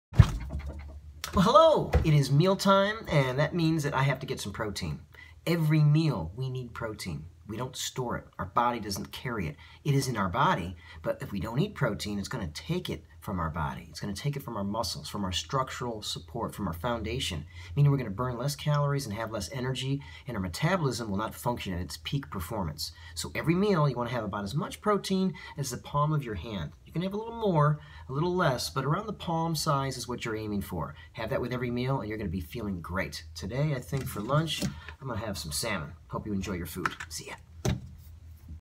Protein with every meal!
Our body needs to ingest protein with every meal, that means breakfast lunch and dinner. We don’t store protein, so if we don’t eat it we end up sacrificing our own bodies muscle and connected tissues that are necessary to be used by her body for metabolism. It’s better not to sacrifice our own bodies tissues and to just eat protein every meal.